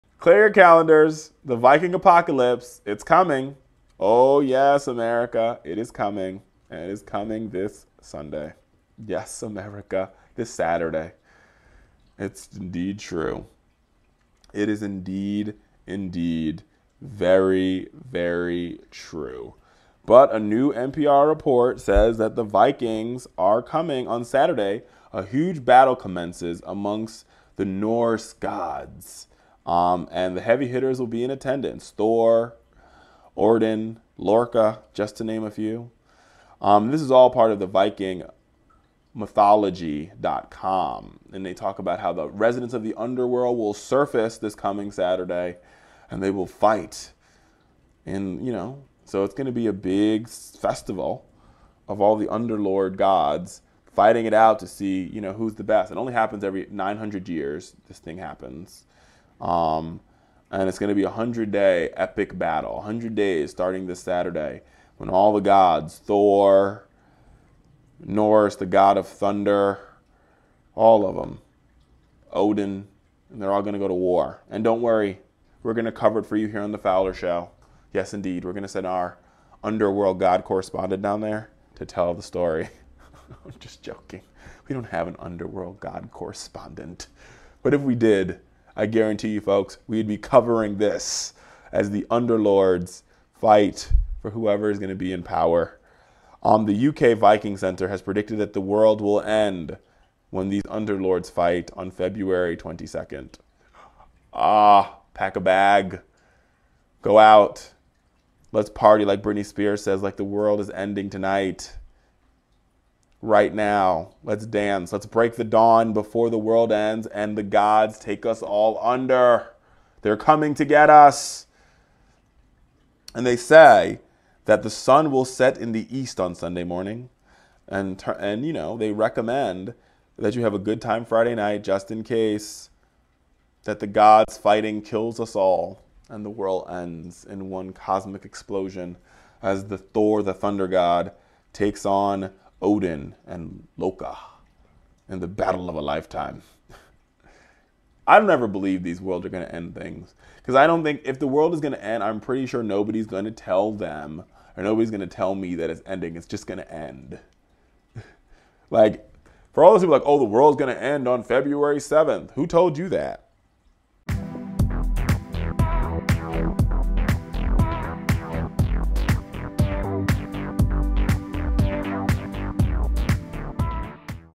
The Viking Apocalypse Is Scheduled For This Month
Now's the time to cash out your life insurance policy and have a little fun, because, according to Norse mythology, the world is ending this Saturday.

Sorry to be a bummer, but you should know we're about 100 days into an epic battle that will culminate with the earth falling into the sea, NPR explains.

On the bright side, at least you'll have an excuse for not going to your nephew's Chuck E. Cheese's party on Sunday.

For those whose understanding of Norse mythology begins and ends with the Marvel Comics' version of "Thor," the story behind Ragnarok, aka the "Viking apocalypse," or "doom of the gods," is arguably more spectacular and imaginative than anything Hollywood could dream up.

Thor, Norse god of thunder (Thinkstock)
Here's what's coming, according to Time magazine. On Saturday a huge battle commences among the various Norse gods. All the heavy hitters will be in attendance — Thor, Odin and Loki to name but a few.

They'll fight (and when gods fight, it gets messy). Eventually, the Earth will split open, Viking-Mythlogy.com explains. Residents of the underworld will surface.

Fortunately, you'll be long gone — meaning all your worries about mortgages and breakups and thinning hair will be but a sweet memory lost to the mists of time. To put a positive spin on things.

NPR reports that in York, England, "Viking enthusiasts" are gathering at the Jorvik Viking Festival for a parade and mock battle. Experts from the Jorvik Viking Centre in the U.K. have predicted the world's end on Feb. 22, the Independent reports.

We're betting the sun will still rise in the East on Sunday morning (the Y2K faux-pocalypse turned us cynical), but we recommend you have a good time on Friday night. Just in case.

3 Steps To Join FowlerNation!
2. 'Like' The Richard Fowler Show on Facebook
3. 'Follow' Us on Twitter